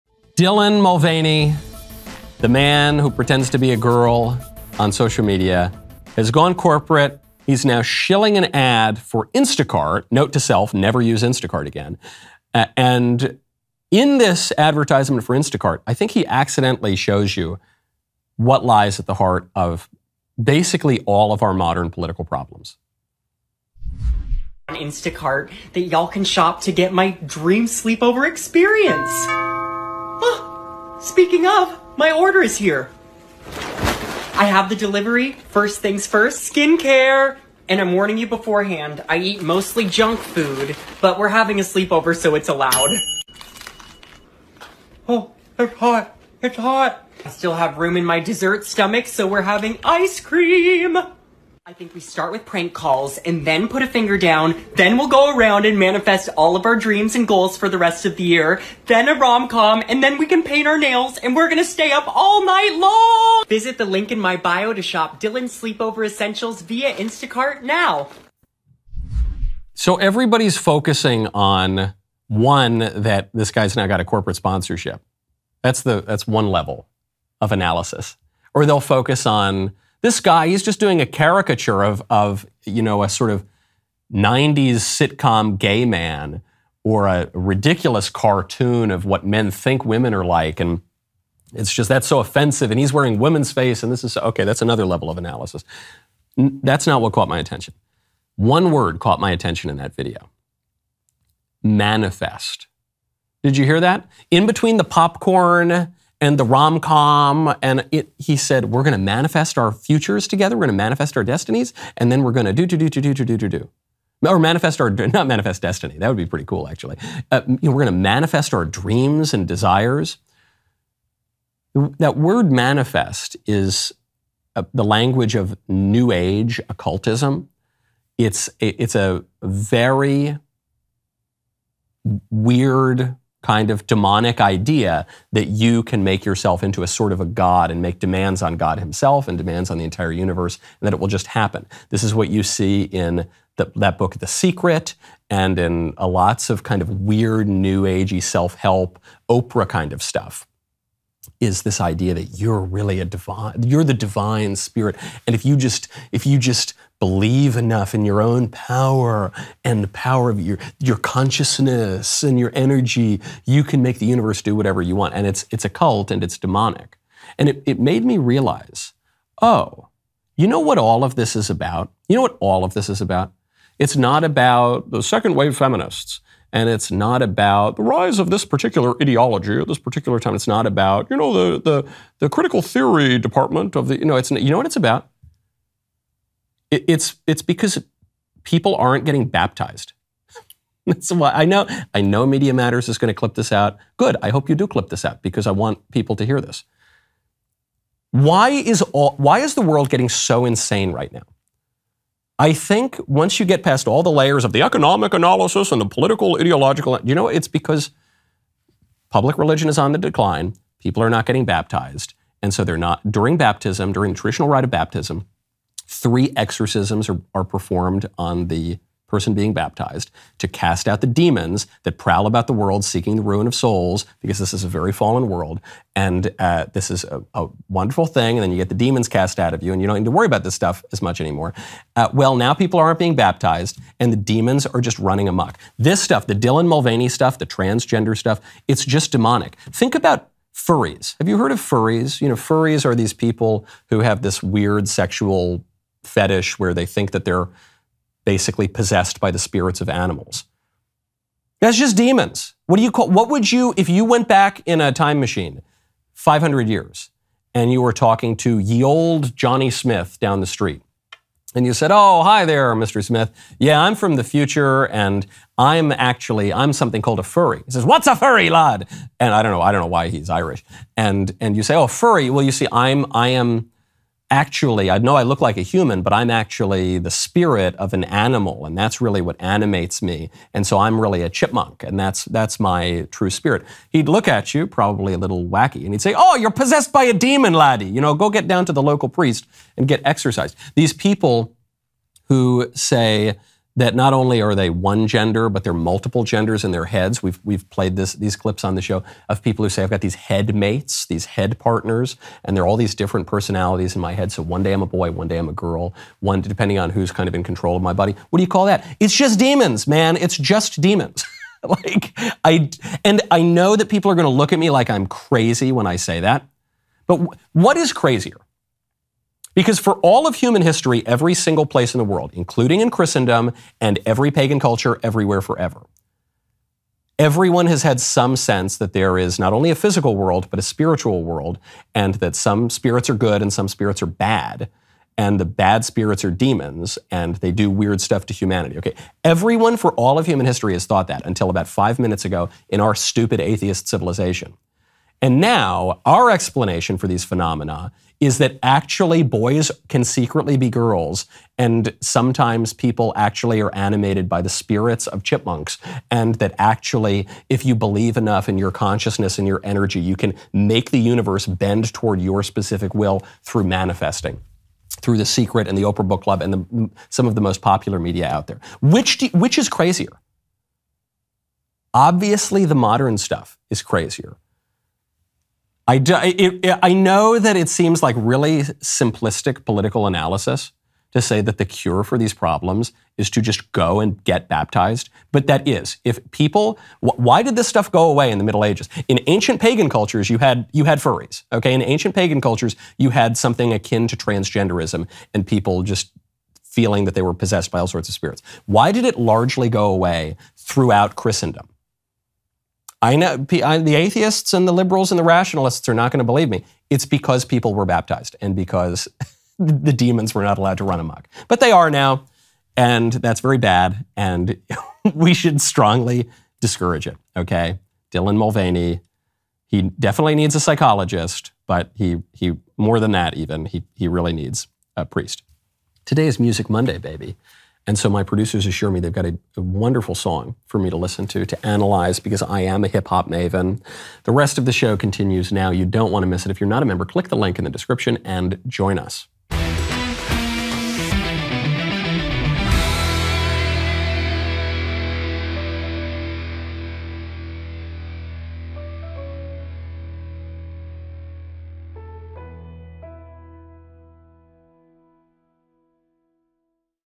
Dylan Mulvaney Needs a Priest
In the latest Dylan Mulvaney news, the transgender TikToker did an ad for Instacart. In the video, he accidentally revealed the root of all of the modern political problems. How do we fix it? Baptize more people.

Don’t dress like a squish.